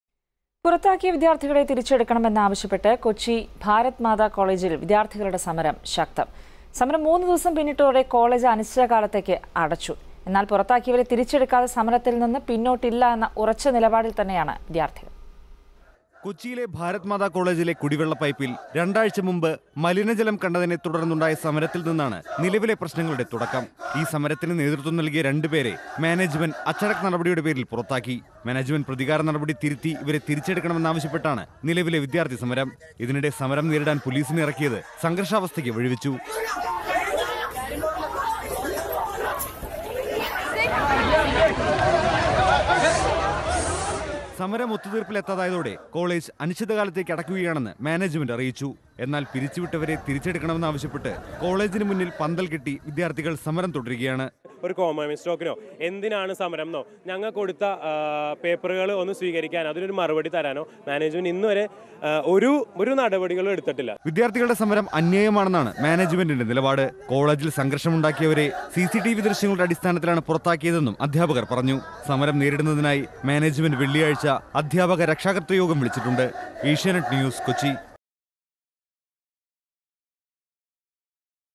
Bharata Matha College students strike , clash between police and students
Bharata Matha College students strike, the clash between police and students. college closed during the strike